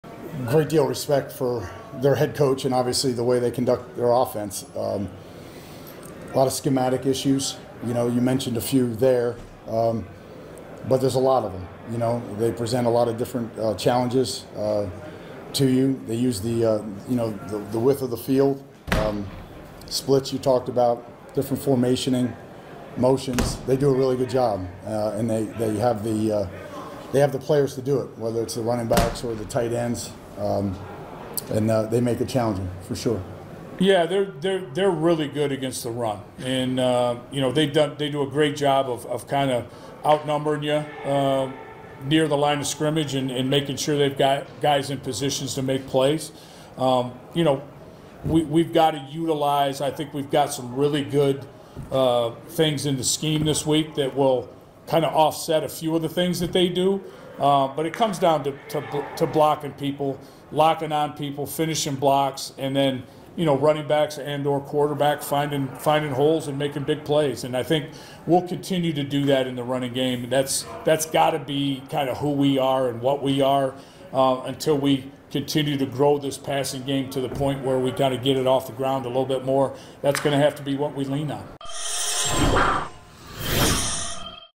Notre Dame Coordinators Al Golden and Mike Denbrock Talk Stanford
The 11th ranked Fighting Irish (4-1) host the (2-3) Stanford Cardinal on Saturday. Kick at 3:30 pm ET.